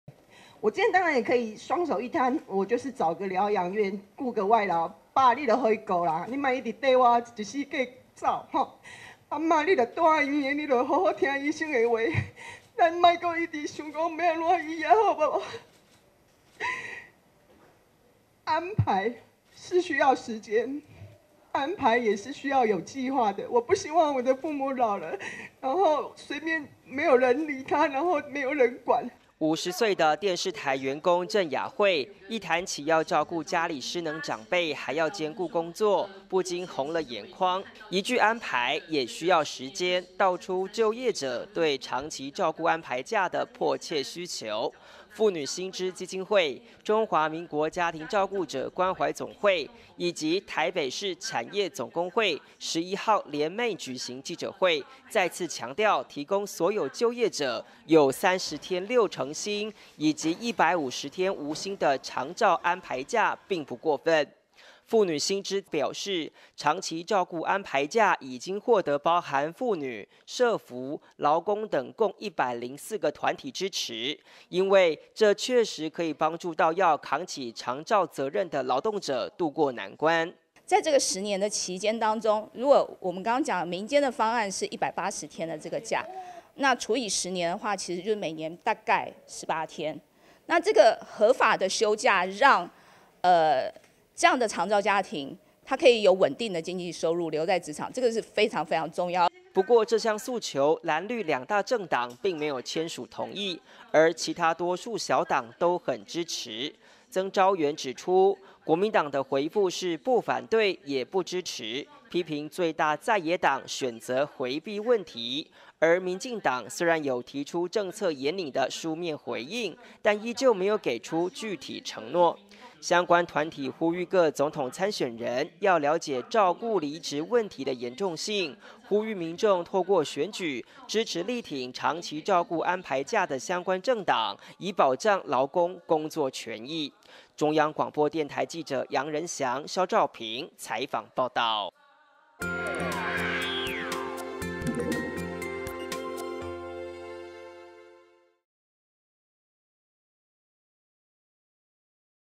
民團指長期照顧安排假有必要 籲總統參選人正視需求【央廣新聞】
包含中華民國家庭照顧者關懷總會等多個民間團體11日共同提出呼籲，指180天的「長期照顧安排假」可以讓身處長照重擔的家庭度過風暴，希望各總統參選人能正視社會確實有「長期照顧安排假」的需求。